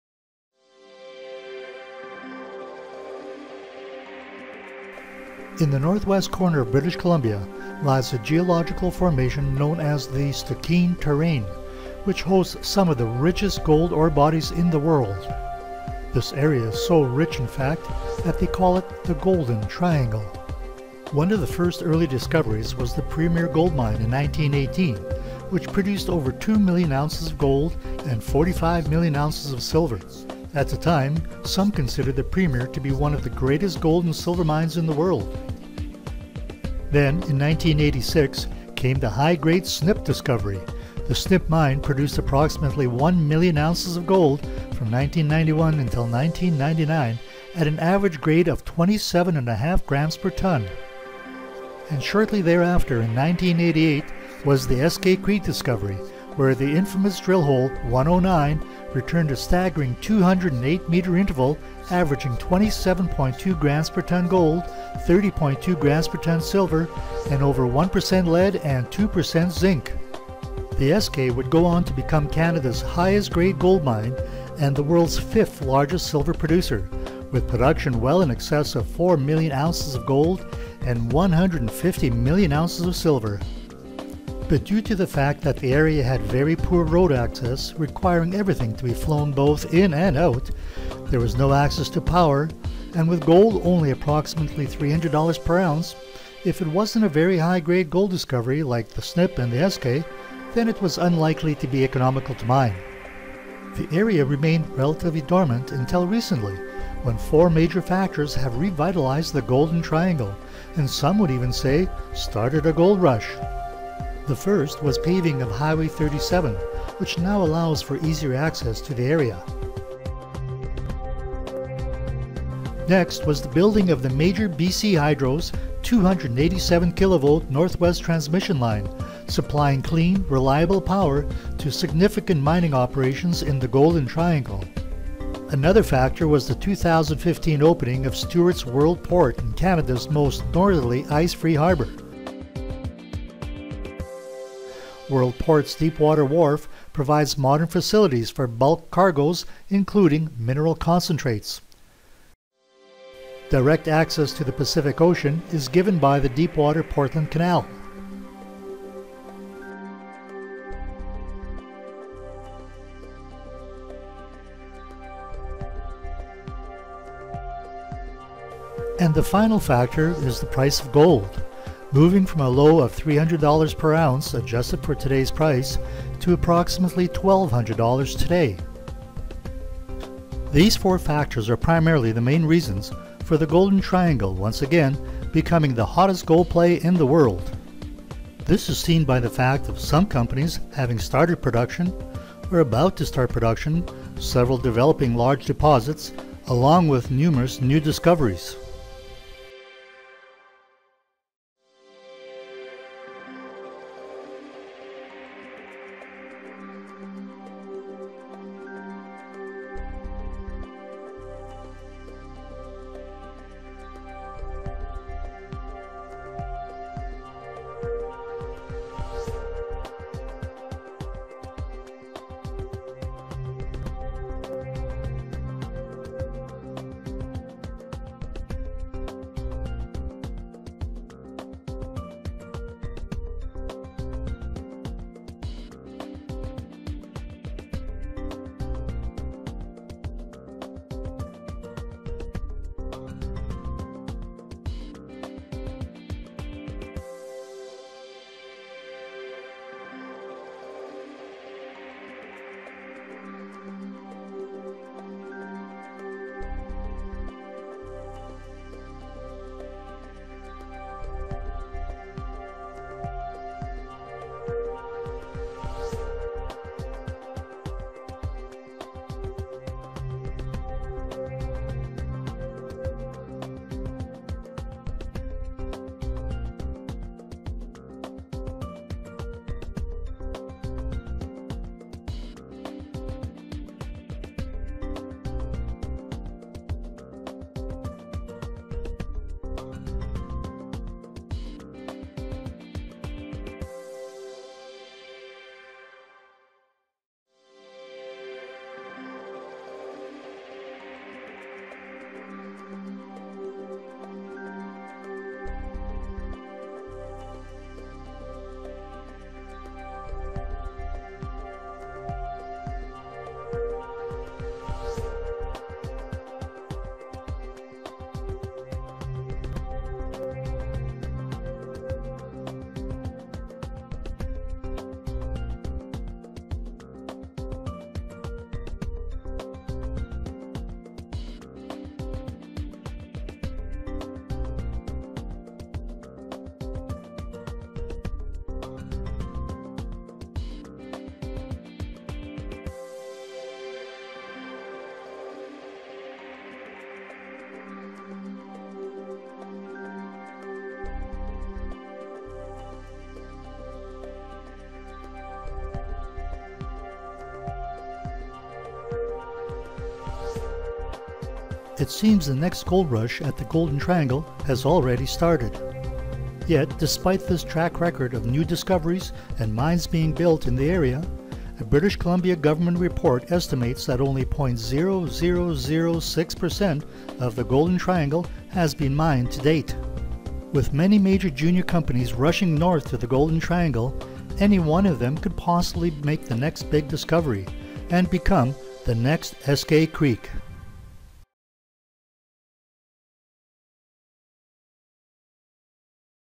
Golden Triangle of British Columbia
The Golden Triangle of NW British Columbia hosts some of the highest grade ore bodies in the world. With the paving of highway 37 providing easier access to the area, the recent construction of a major power line and the recent opening of a nearby deep sea port enabling easy shipping of ore and of course the relatively higher price of gold ($1,200/oz vs $3,00/oz) the Golden Triangle has attracted a host of resource companies all vying to be the next big gold producer.